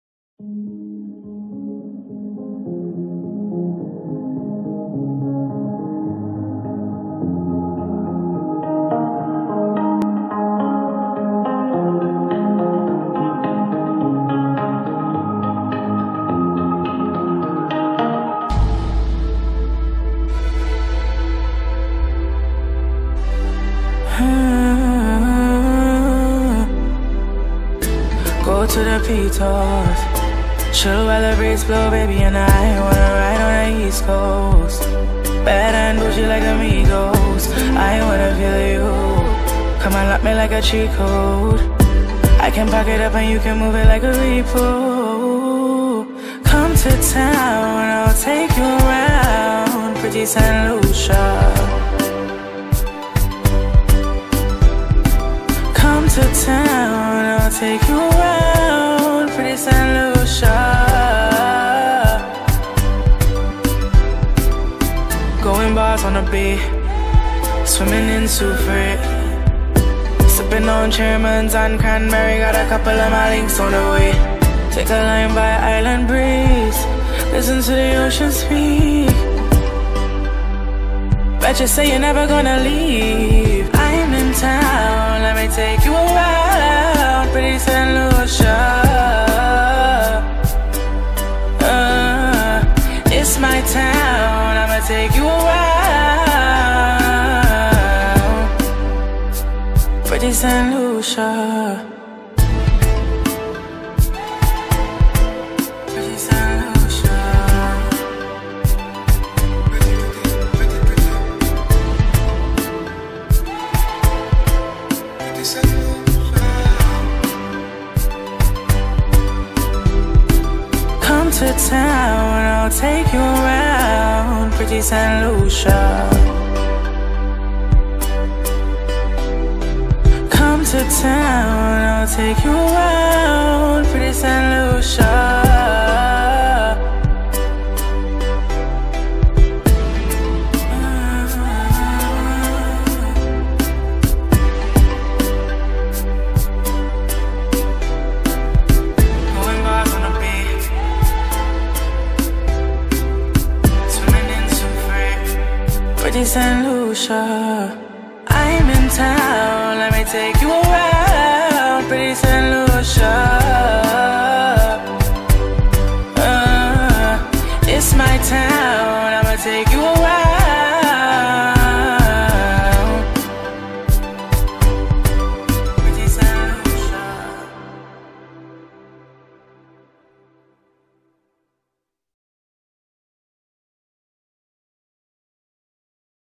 Snorkeling with Island Taxi & Tours
Mini highlights of snorkeling activity here in St. Lucia, so much to see and so much to do, interestingly on the west coat of St. Lucia as well as scuba diving areas. Feel free to have look at this mini video with our Touring and Taxi company. Find us on Face book @758 island taxi & tours and on instagram @758islandtaxi

Thanks for supporting and recommending